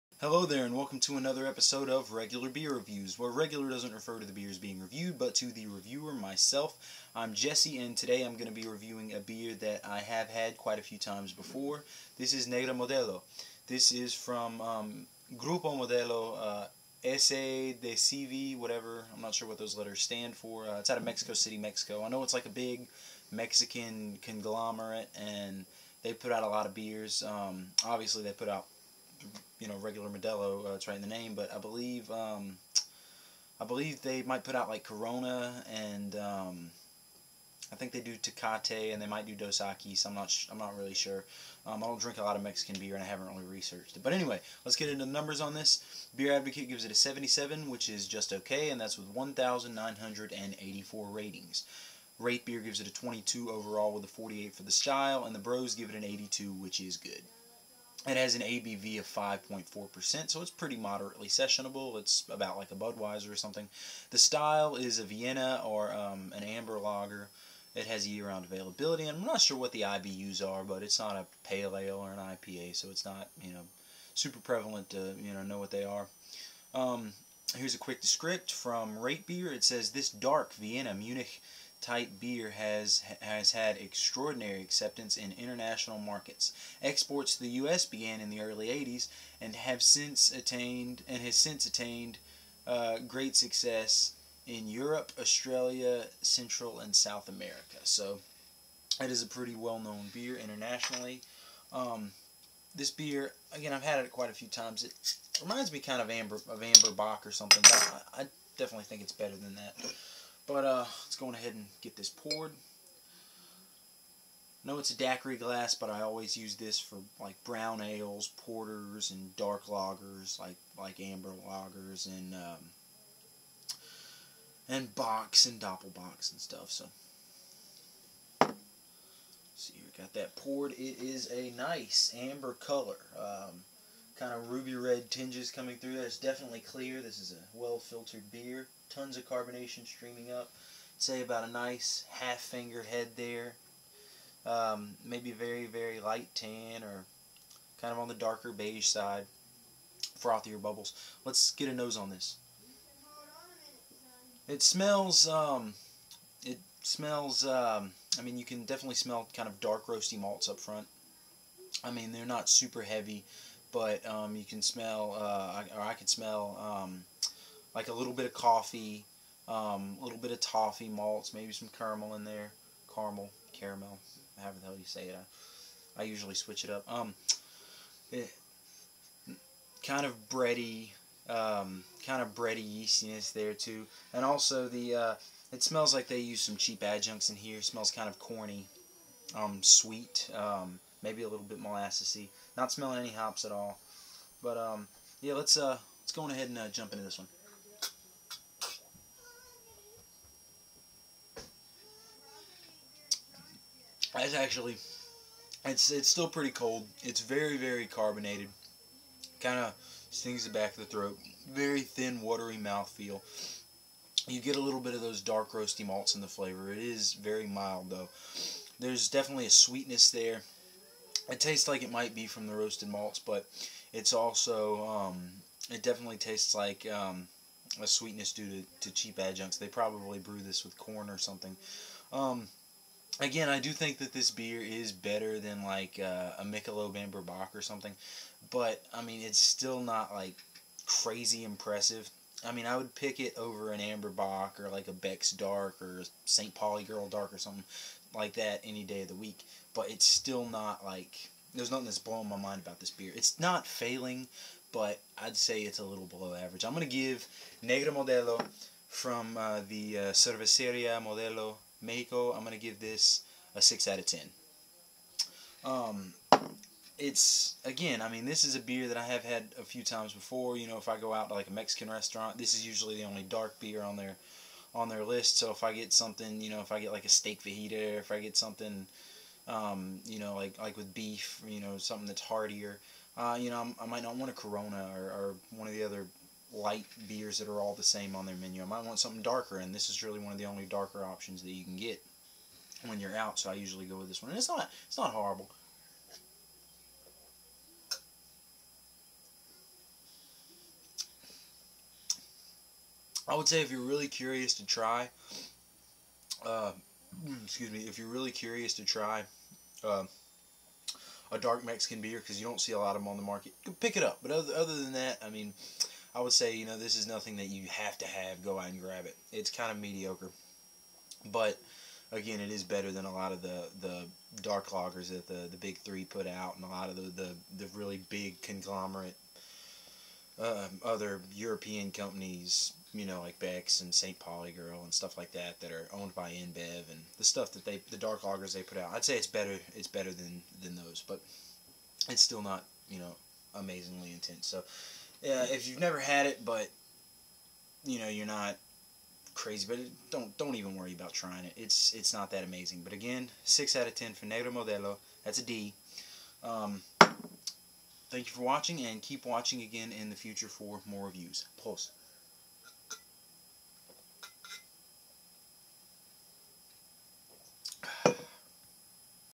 RBR - Negra Modelo
This is Negra Modelo. It is a Vienne Lager from Grupo Modelo out of Mexico City, Mexico.